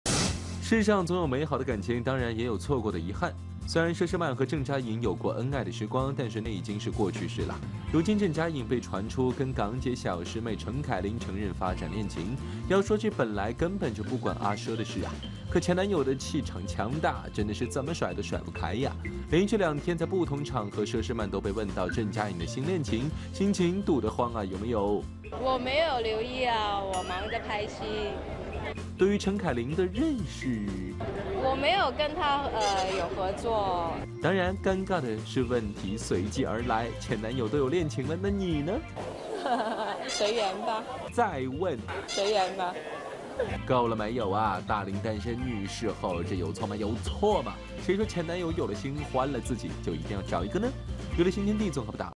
《看看星闻》:佘诗曼Charmaine Sheh懒理郑嘉颖Kevin Cheng闹“父女恋” Kankan News【SMG新闻超清版】
佘诗曼5月10日出席“点滴背水行”活动，适逢母亲节，佘诗曼透露晚上将与家人吃饭庆祝，问到是不是有时间拍拖?她笑着说：“我现在还没有拍拖，只是休息时间多了，放假也想跟妈妈去旅行。”